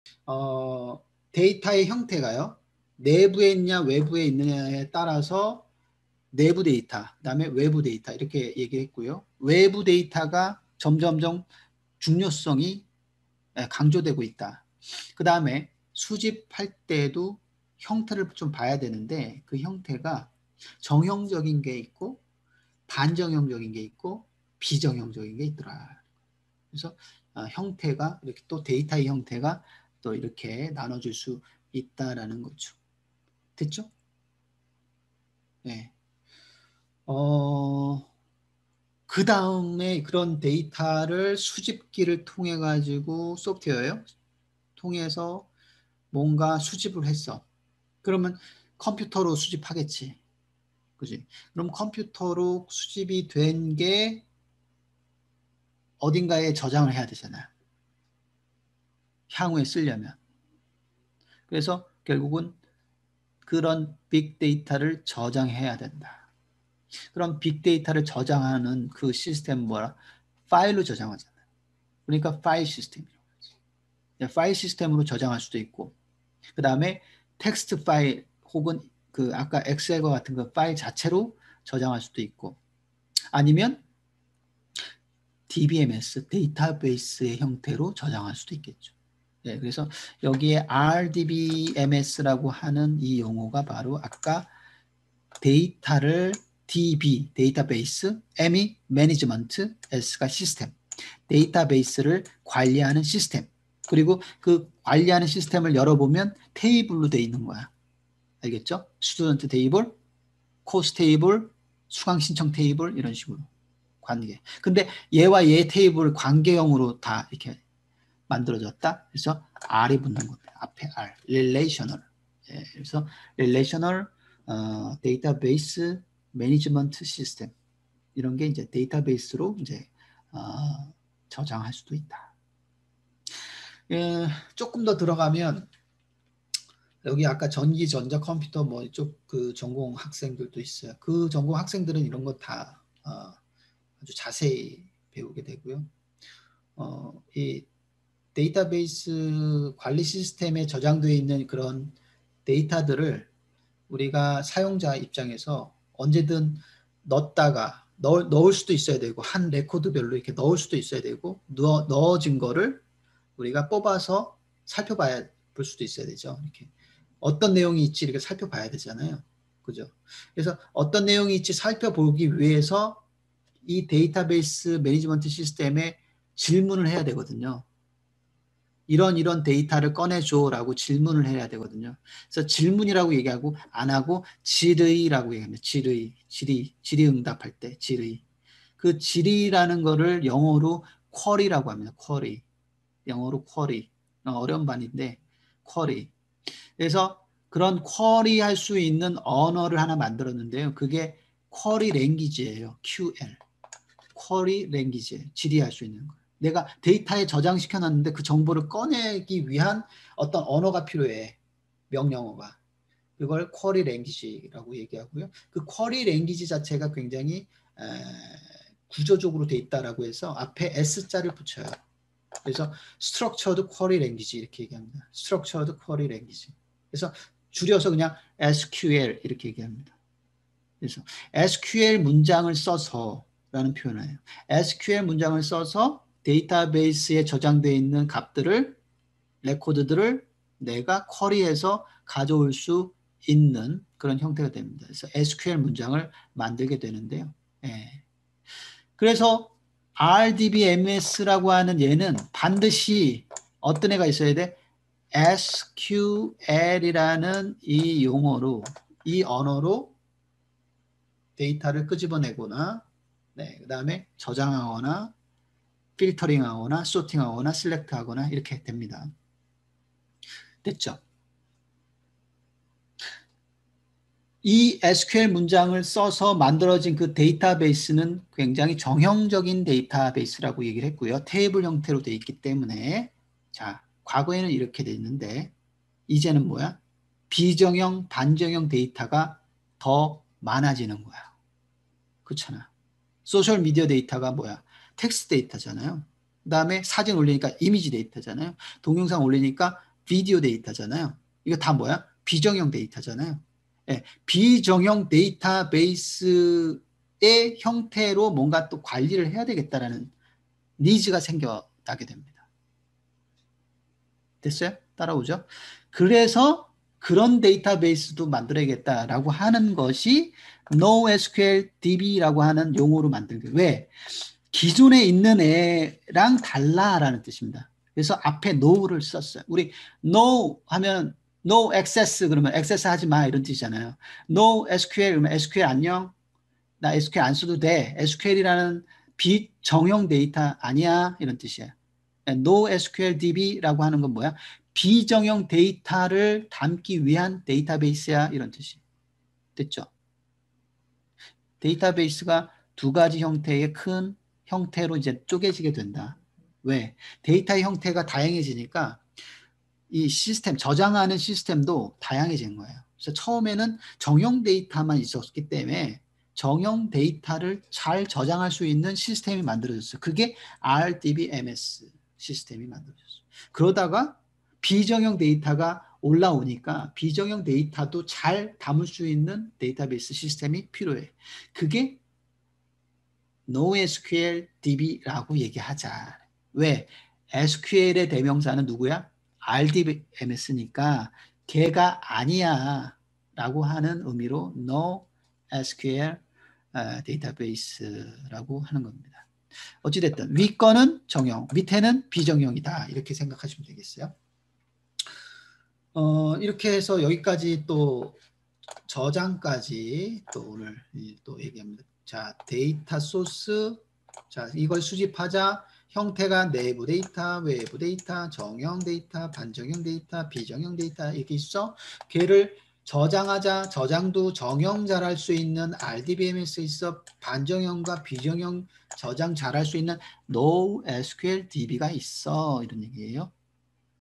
빅데이터를 수집해서 어떻게 저장하면 될까요? 데이터의 형태가 다양해지니깐 데이터를 저장하는 시스템도 다양해져야 합니다!!
빅데이터 강의 노트